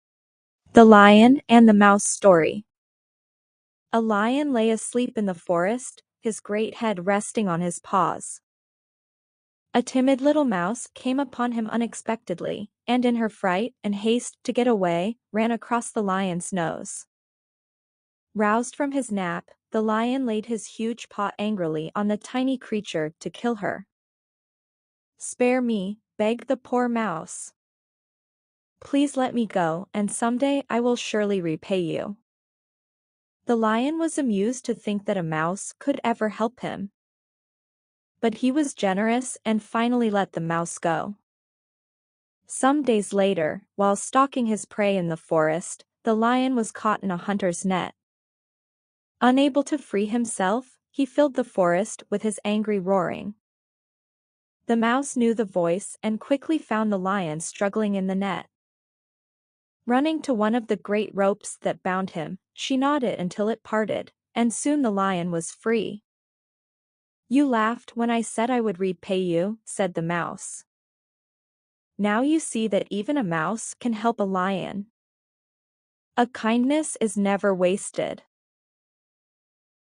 The Lion and the Mouse story
Experience the classic fable of The Lion and the Mouse in this heartwarming and timeless story. Join the brave mouse as she outwits the mighty lion and learns the value of kindness and friendship. This beloved tale is perfect for children of all ages and will leave a lasting impression on readers. Allow yourself to be transported to the animal kingdom and witness the power of compassion in action.